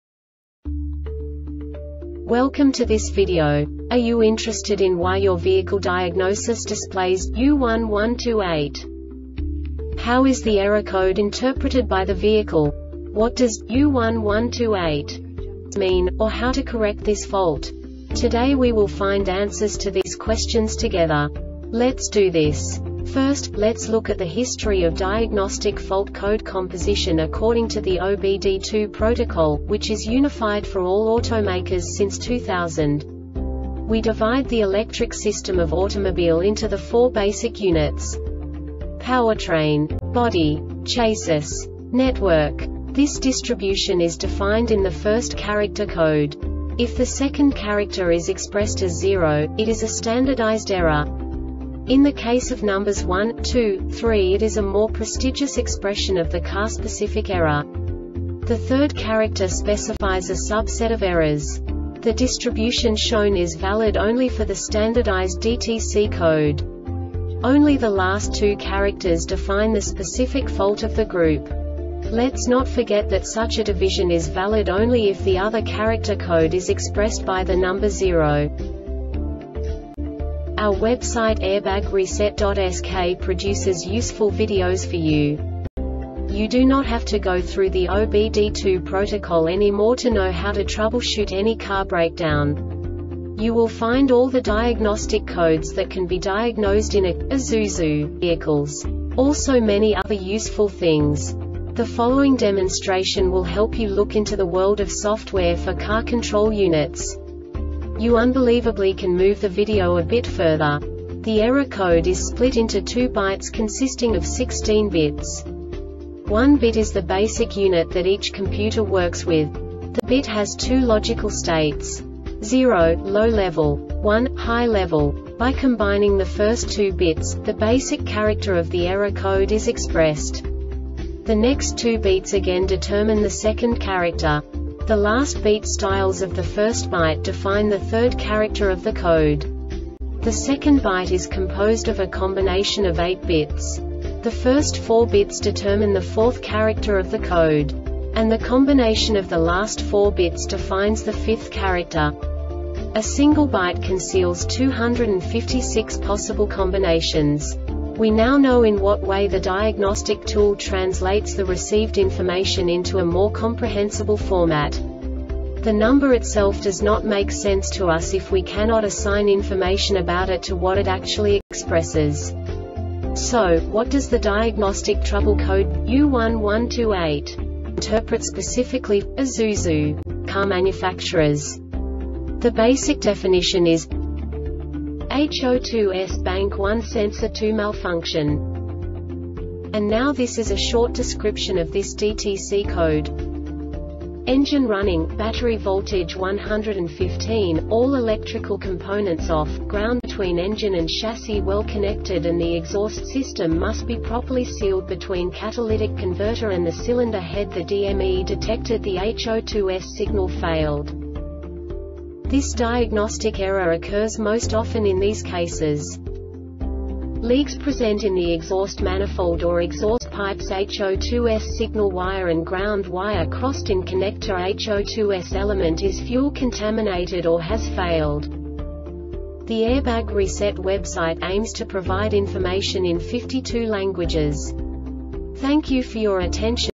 DTC Isuzu U1128 Short Explanation
Contents:
0:21 Basic DTC analysis according to OBD2 protocol standard.
1:48 Insight into programming
2:58 A diagnostically understandable description of the Diagnostic Trouble Code
3:05 Basic error definition

Make:
Isuzu
Code:
U1128
Definition:
HO2S (Bank 1 Sensor 2) Malfunction
Description:
Engine running,battery voltage 11.5, all electrical components off,ground between engine and chassis well connected and the exhaust system must be properly sealed between catalytic converter and the cylinder head.The DME detected the HO2S signal failed
Cause:
Leaks present in the exhaust manifold or exhaust pipesHO2S signal wire and ground wire crossed in connectorHO2S element is fuel contaminated or has failed